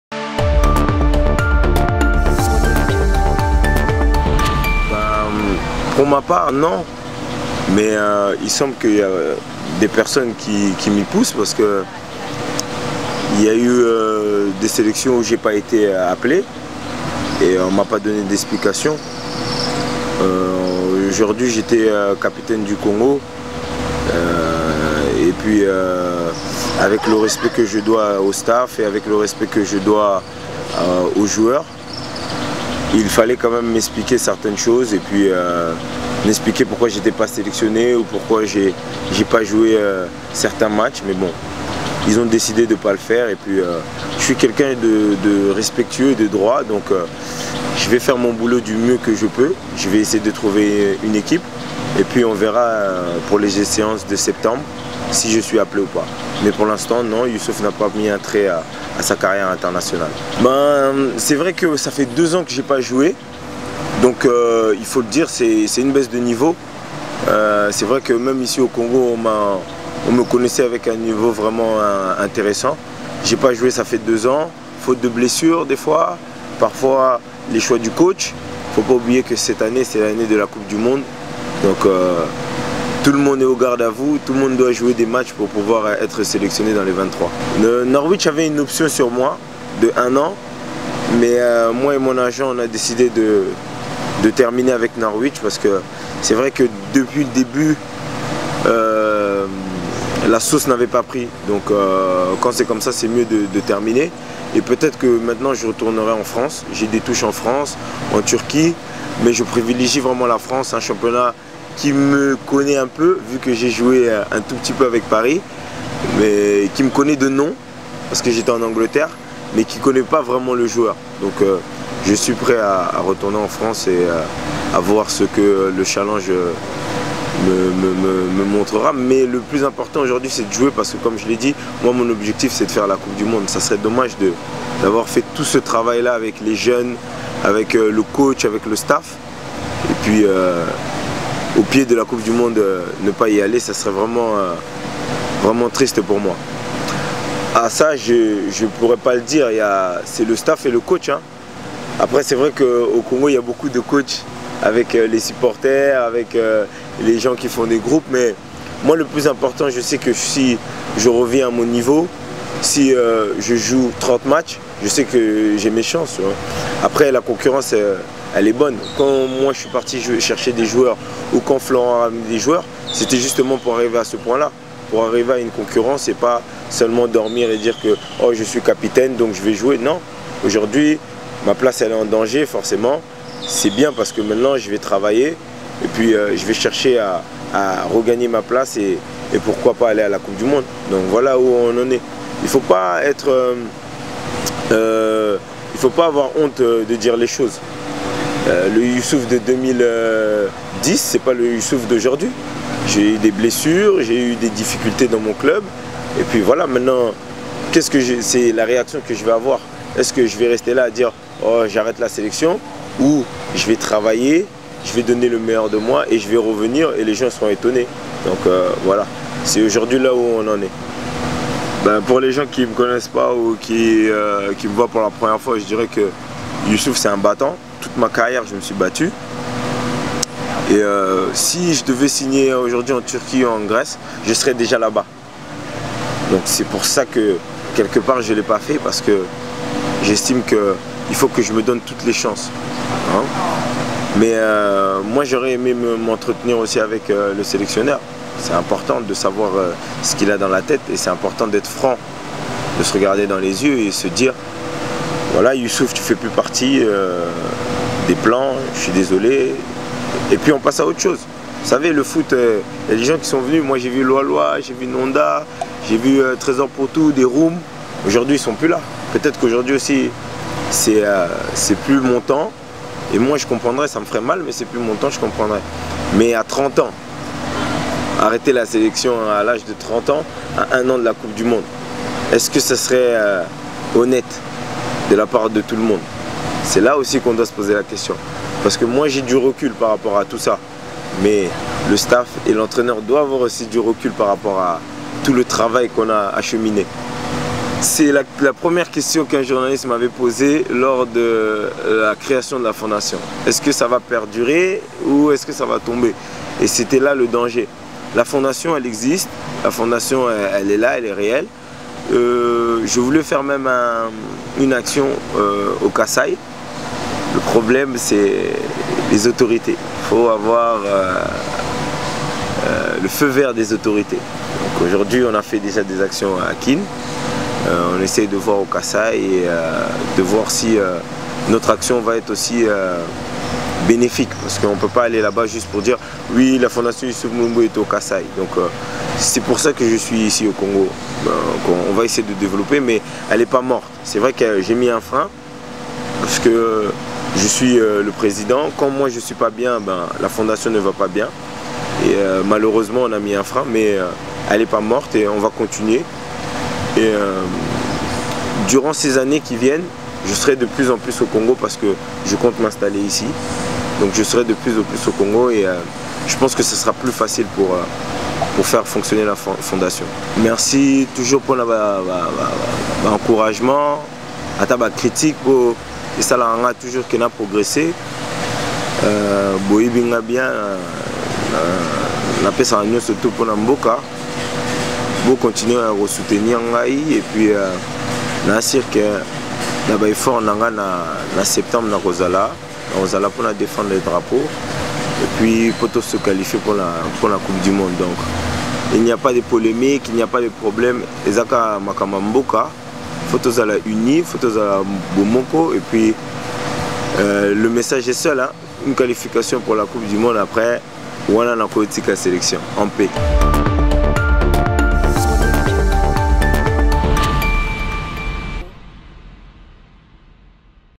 Youssouf Chafiq Mulumbu Ngangu (HD)
Après une saison 2016—2017 difficile et compliquée, Youssouf Mulumbu revient sur sa saison, ses blessures, la sélection nationale, son avenir de footballeur et sa vie privée.